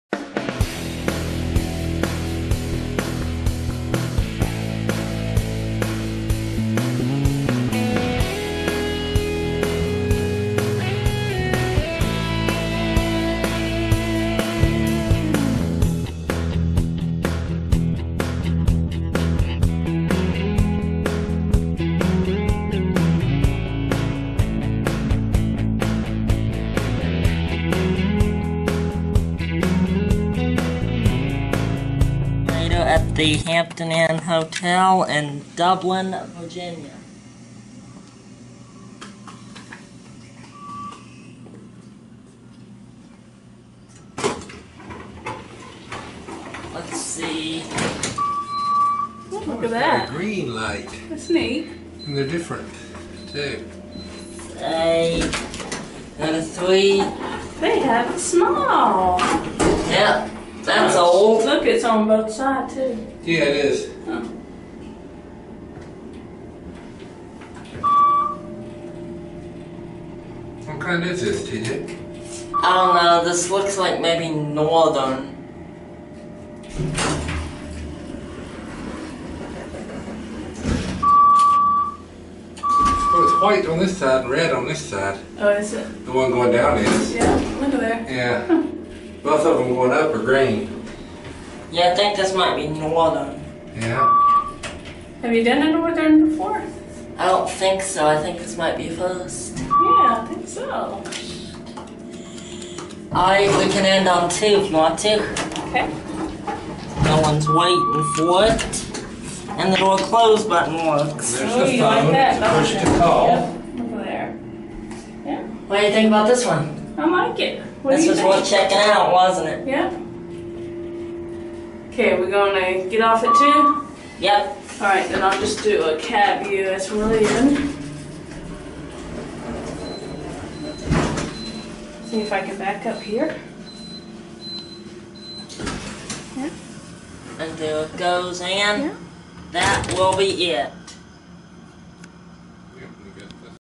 Thyssenkrupp Northern Elevator at the Hampton Inn in Dublin, VA
We stopped here on our way to Roanoke to meet up with Dieselducy & TheElevatorChannel Very nice Hampton which is in the middle of renovations. It was nice to meet Cassidy at the front desk who told me he was one of my YT subscribers. He also mention he's a subscriber to Dieselducy & Cannycart's as well. I believe the elevator is a ThyssenKrupp Northern Elevator. I have more photos we took on my FB